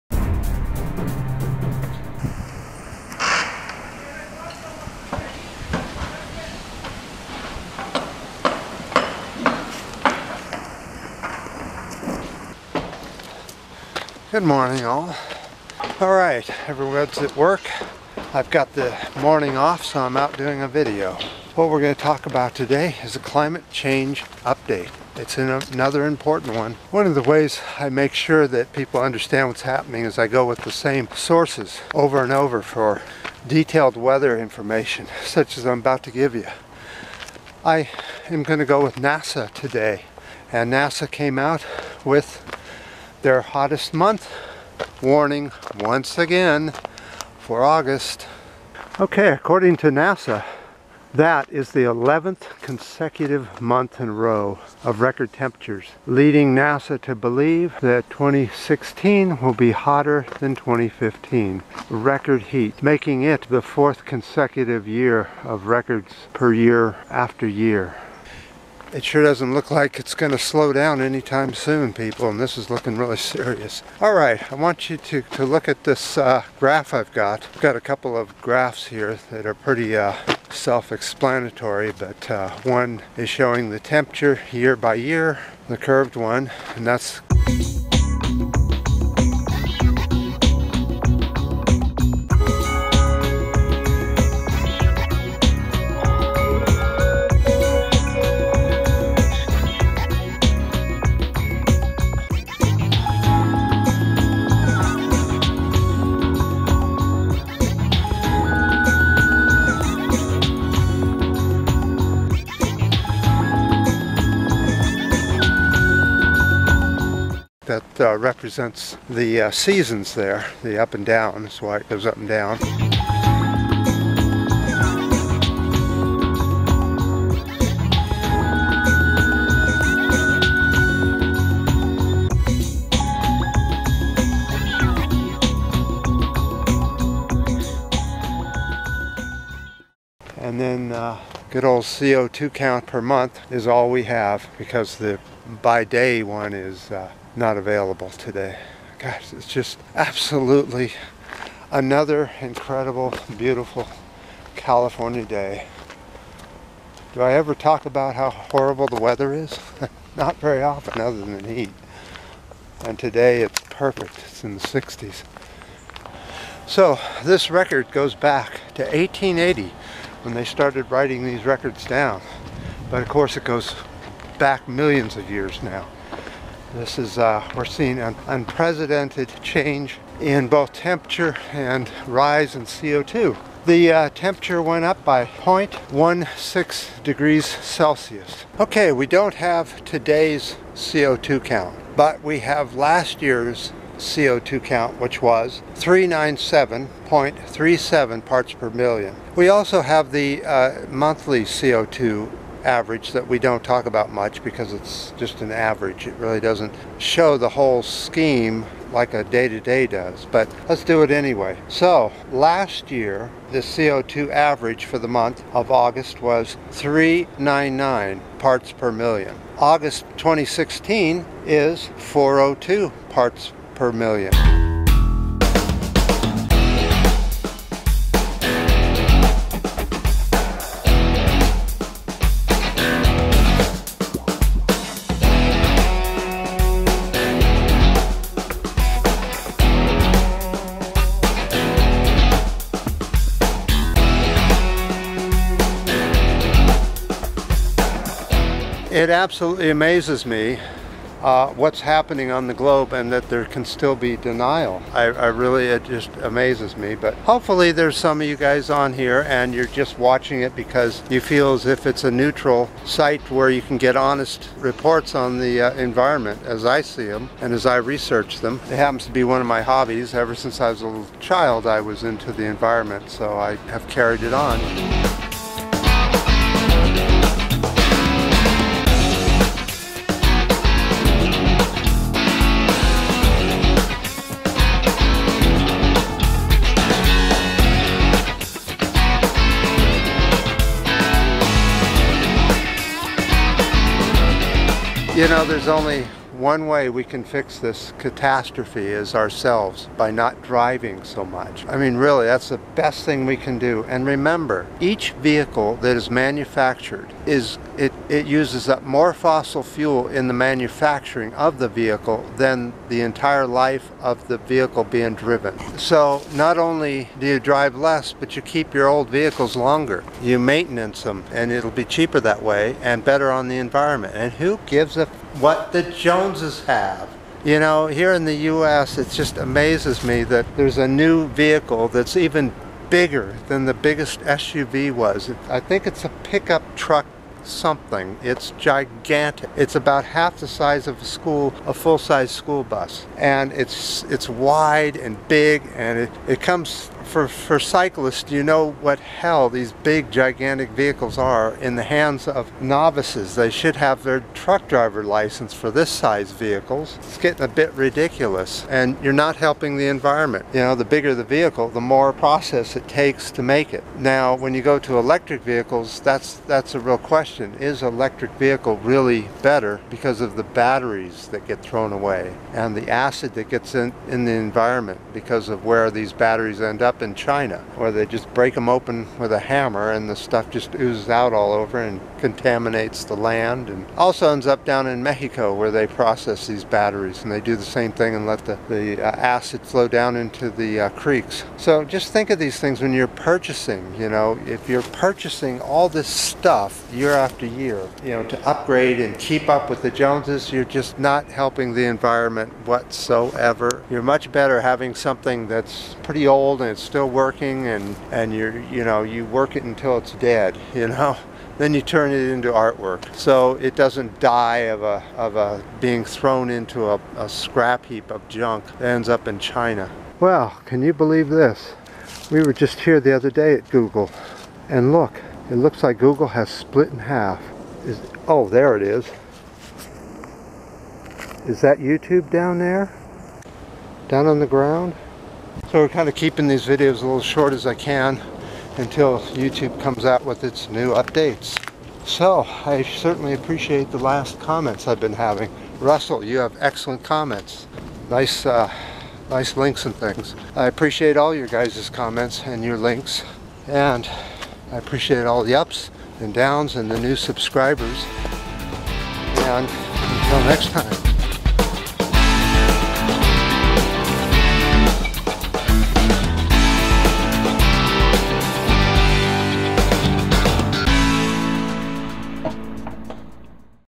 Climate Change Update For August 2016
Here is the Climate Change Update For August 2016 with Co2 count and more.